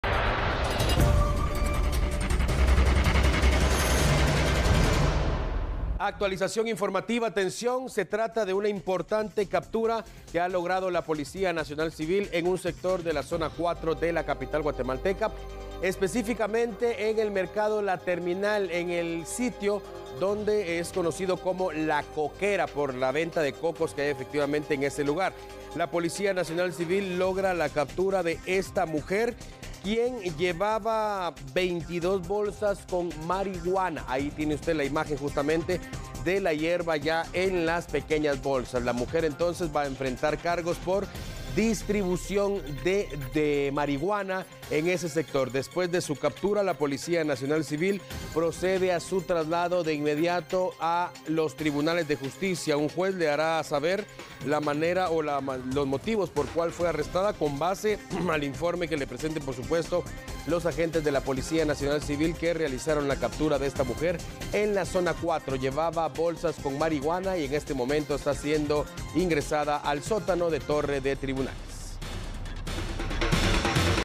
Capturan a presunta distribuidora de drogas en la Zona 4 capitalina
Agentes de la Policía Nacional Civil lograron capturar a una presunta distribuidora de drogas, quien fue sorprendida con 22 bolsas de marihuana que comercializaría en sectores de la Zona 4 capitalina.